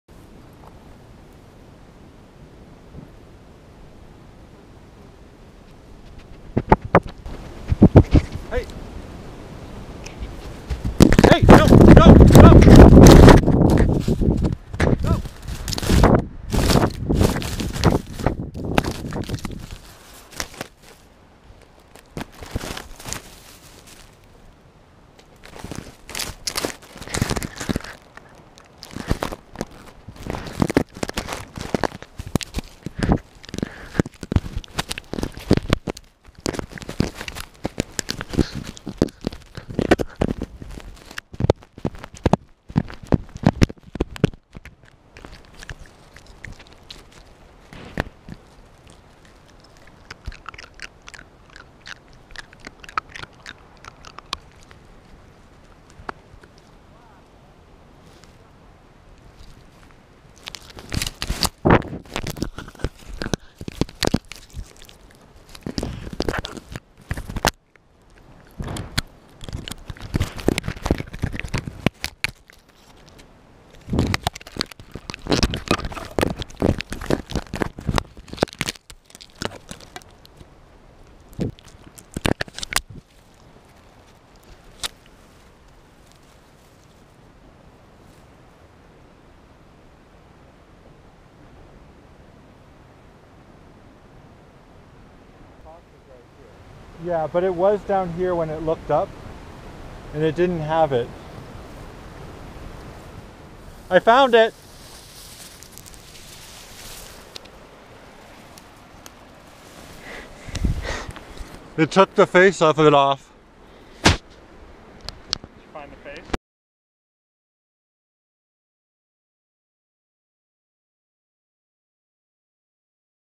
FOX KILLS AND EATS my gopro
Last week a curious fox found and then ran off with (and seriously injured) my gopro.

The story: I went to Round Island, Alaska, to film wildlife with the University of Alaska Anchorage.  My friends were photographing and filming sea lions when I spotted this fox come along.  So I stupidly put my gopro on the ground in hopes of getting a close-up.  When the fox ran off, I thought I'd never see my camera again.  For about 8 minutes we looked for it, then there it was!  It turns on and records, but the lens is messed up and I'll need to have it repaired.  I'm glad the fox itself didn't get hurt or swallow anything that could have harmed it.

Thanks to my friends who were there with me: Travis Rector, Paola Banchero, Adrienne Lindholm, and Henry & Leslie Need.

More about our project: We went to Round Island to acquire footage in order to make a fulldome planetarium show about this special place where hundreds and sometimes thousands of walruses haul out on the beach, sea lions play in the surf, 250,000 sea birds nest, and some 30 foxes live.  It's an amazing island with a long, long natural and cultural (Yup'ik) history.  To learn more about the project, please